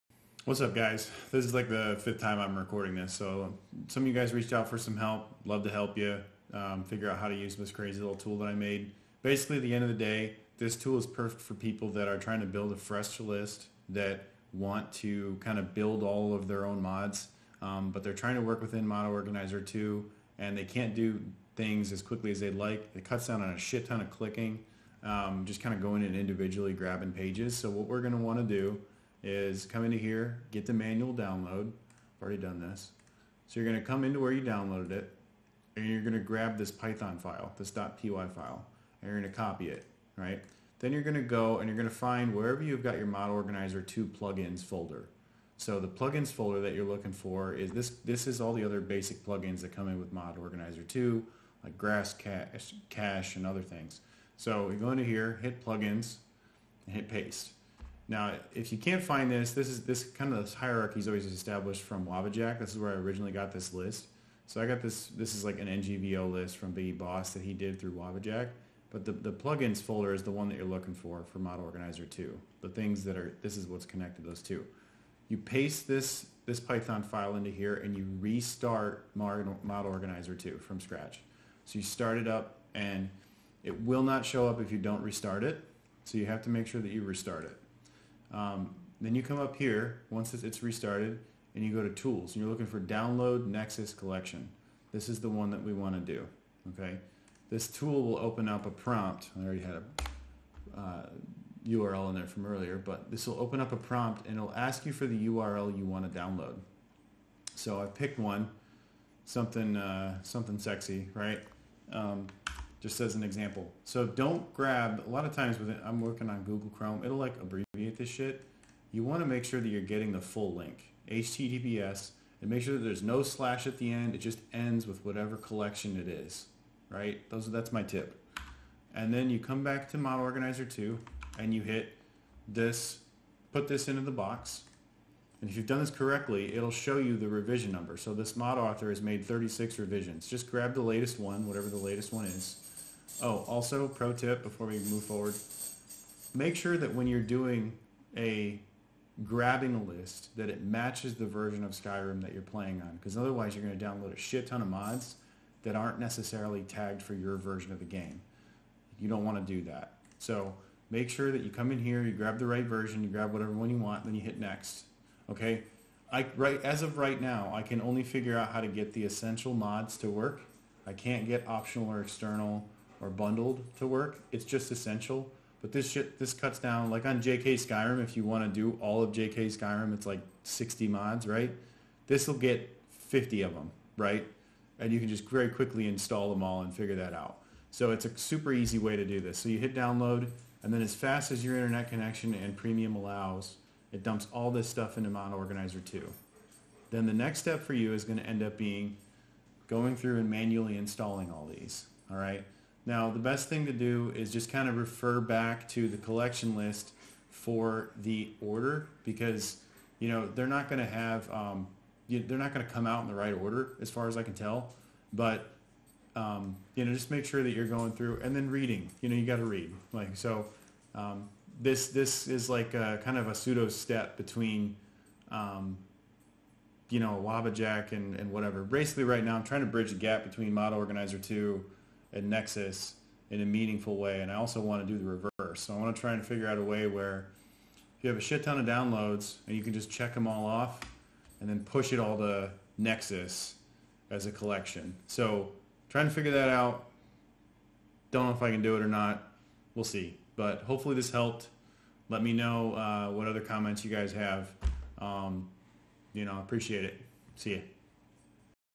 MO2 Collections Download Tutorial
🎮 Nexus Collections Downloader for Mod Organizer 2

Tired of manually downloading mods from Nexus Collections into MO2?
This plugin makes it easy. Paste a link, pick your revision, and let it grab every essential mod for you—right inside Mod Organizer 2.

🔧 What it does:
✅ Paste any Nexus Collection URL
✅ Choose a revision
✅ See a breakdown of essential, optional, external, and bundled mods
✅ One-click download for all essential mods
✅ Built-in GUI and full MO2 integration
✅ More features on the way!

🛠️ Requirements:
• Mod Organizer 2 with Python plugin support
• Nexus Premium account
• Logged in to Nexus Mods in your browser

🧠 Credits:
Original plugin by Kojillama
Updated & extended by Drew + Eva

💬 Drop your feedback in the comments and let us know what features you'd love to see next.